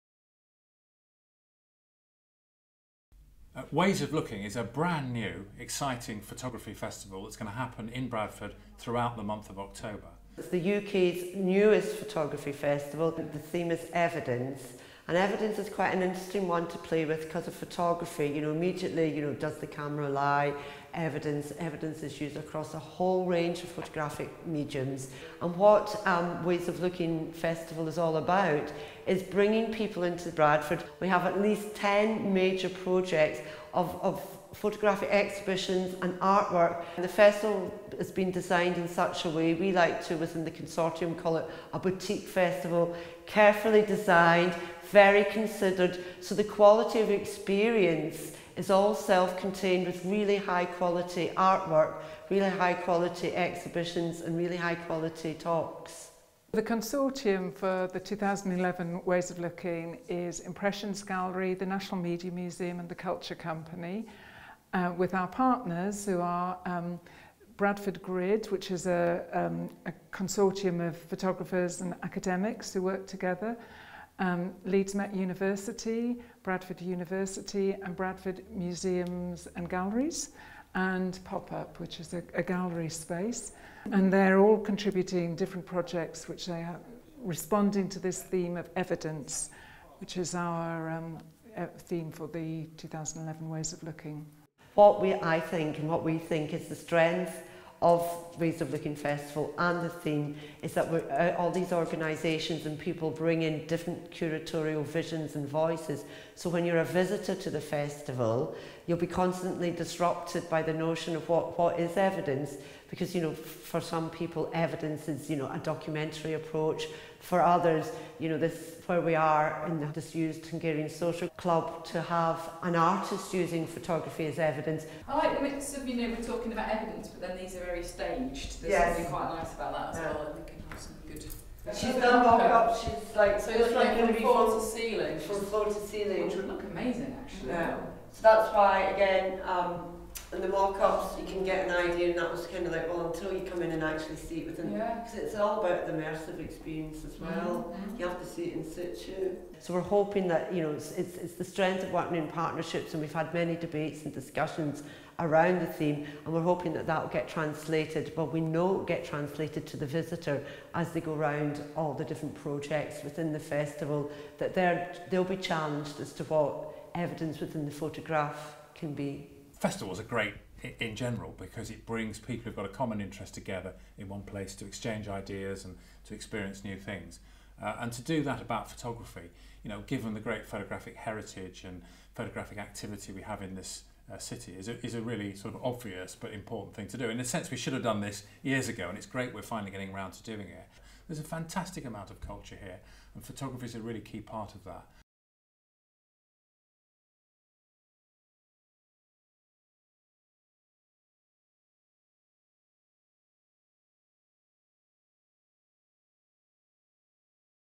Ways of Looking Photography Festival 2011
Ways of Looking is a new festival of photography in Bradford and runs throughout October 2011. Exploring the theme EVIDENCE, the festival presents new exhibitions and commissions, photography in public spaces, collaborations with Bradford residents, a specially published book, and an inspiring programme of events. The festival is funded by Arts Council England through the National Lottery Fund